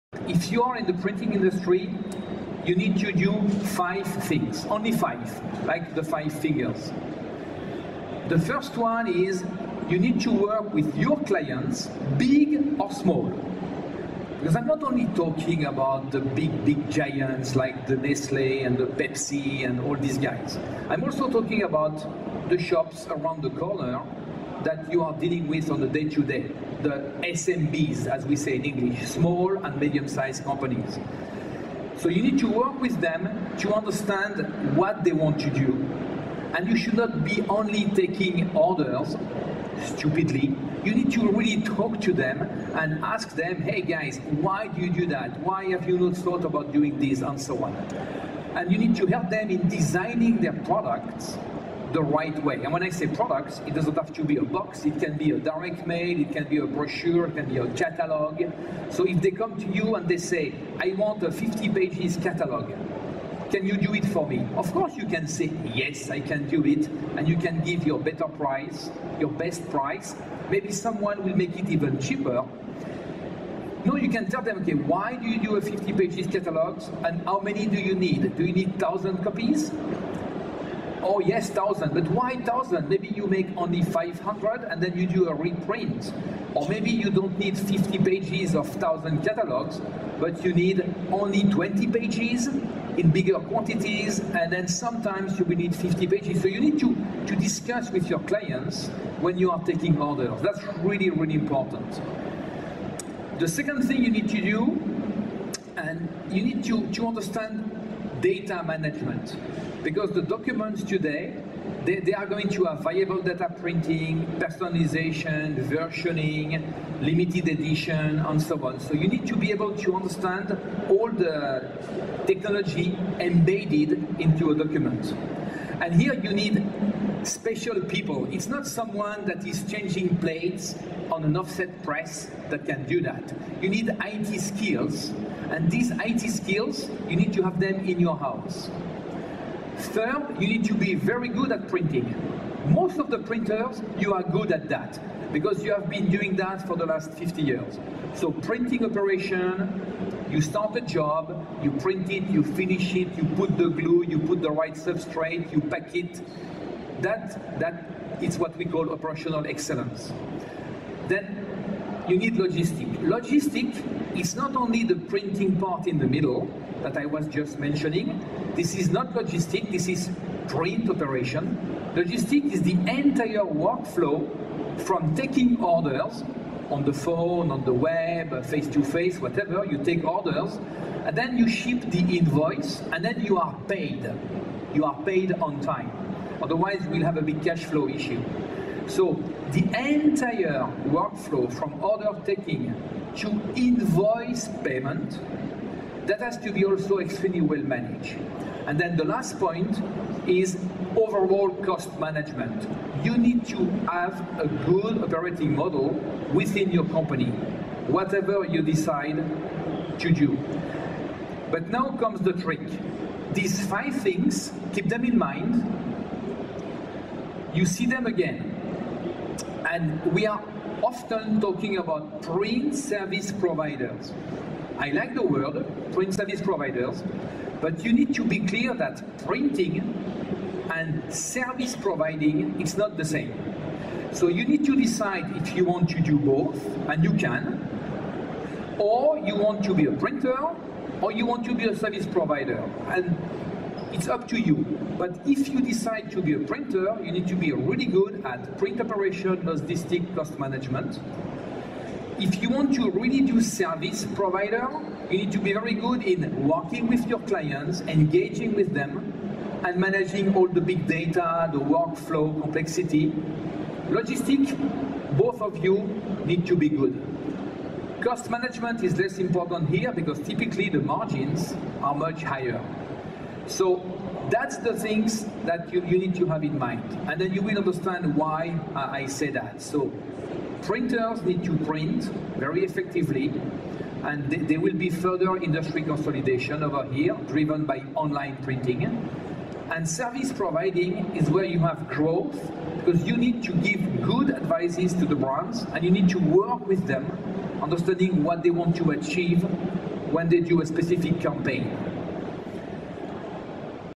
Printers / Service Providers - Francois Martin
Extract from Keynote at IPEX 2017. Activities for both printers and service providers. Martin suggests a choice is needed. Service providers closer to customers, understand data, have higher margins. Printers control costs and scale.